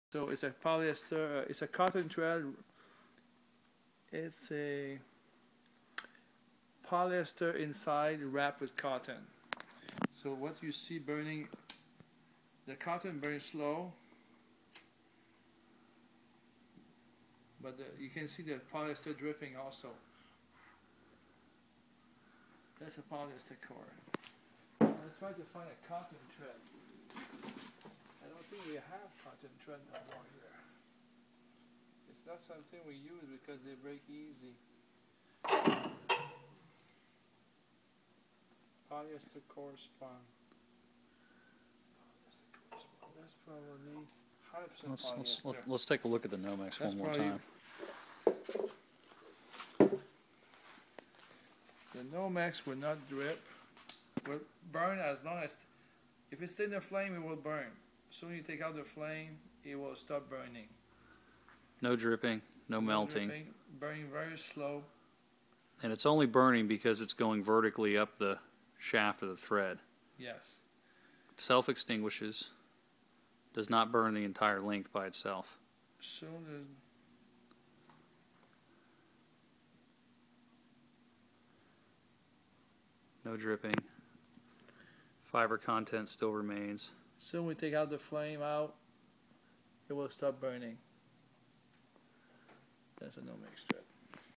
Polycotton vs Nomex 1.MOV
Flame resistant thread vs a non flame-resistant polyester cotton thread.